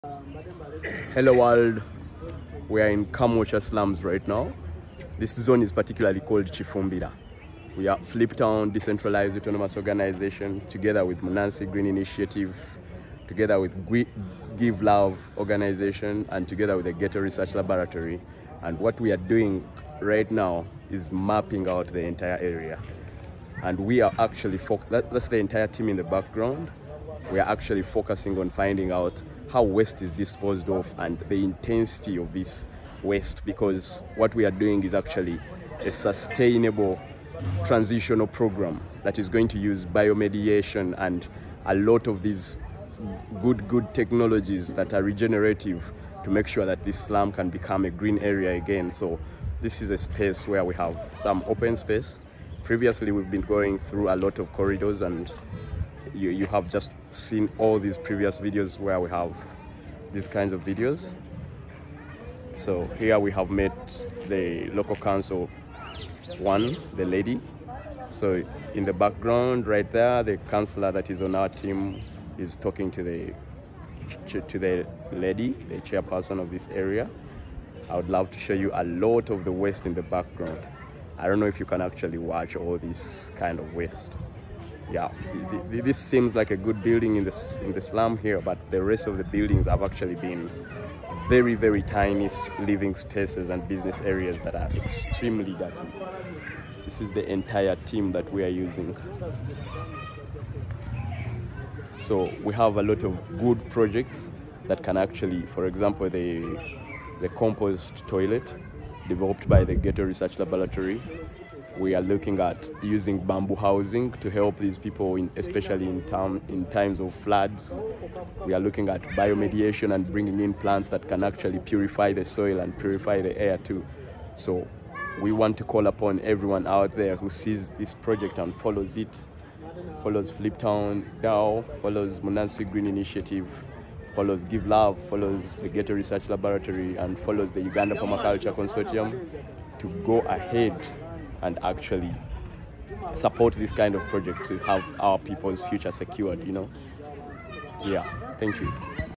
Sustainable Solutions in Uganda's dirtiest slums
Fliptown DAO Partners with Ugandan Organizations for Sustainable Solutions in Kamwokya

Fliptown DAO, a decentralized autonomous organization (DAO), is in a  collaborative effort with several Ugandan organizations to develop sustainable solutions for the Kamwokya community, a slum area within Kampala.

This partnership brings together Fliptown DAO's resources with the local expertise of Give Love, the Ghetto Research Laboratory in Kamwokya, Munansi Green Initiative, and the Uganda Permaculture Consortium. By working together, these groups aim to co-create solutions that directly address the needs and aspirations of the Kamwokya community.

The involvement of organizations focused on community involvement, social welfare, environmental sustainability, and permaculture suggests a multifaceted approach. This encompasses projects like:

- Community gardens and urban agriculture initiatives to promote food security and access to fresh produce.
- Waste management and recycling programs to improve sanitation and environmental conditions.
- Educational workshops and training programs to equip residents with skills for sustainable living and income generation.
- Social enterprise development to create economic opportunities within the community.

Fliptown DAO's involvement signifies the potential for blockchain technology and tokenisation to play a role in facilitating these projects. The collaboration presents an exciting opportunity for Kamwokya and demonstrates a promising approach to sustainable community development in Uganda.